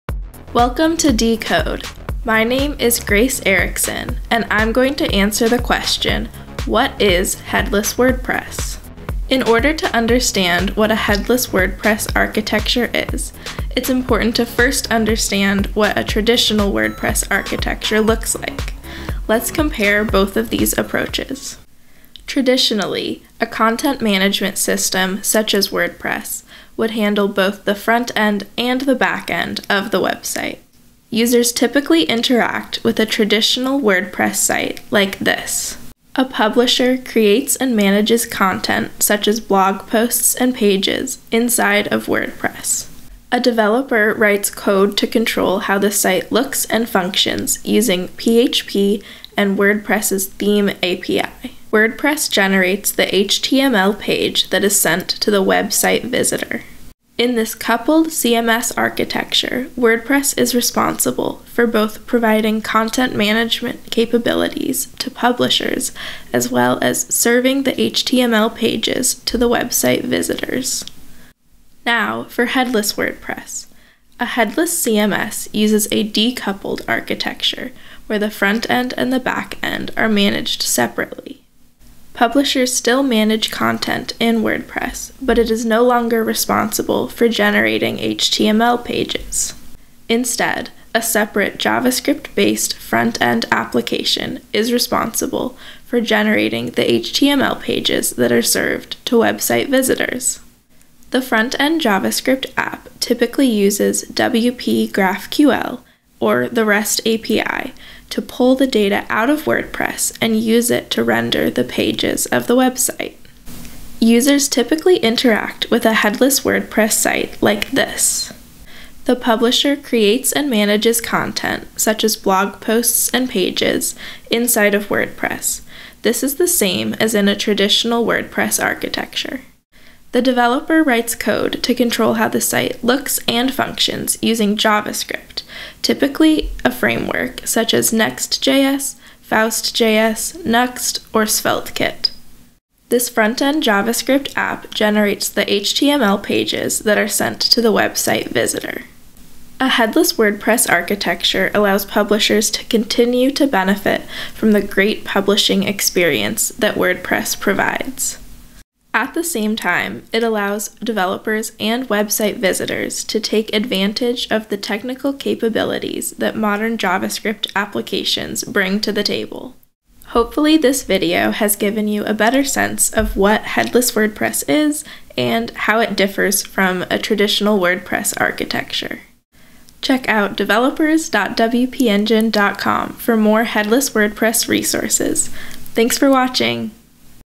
What is Headless WordPress?
Let's talk about what a headless WordPress architecture is and how it’s different from a traditional WordPress architecture. A blog post on this topic is available

CONTENTS:
00:00 - Intro
00:21 - Traditional WordPress Architecture
01:11 - Headless WordPress Architecture
02:49 - Outro

for more headless WordPress content!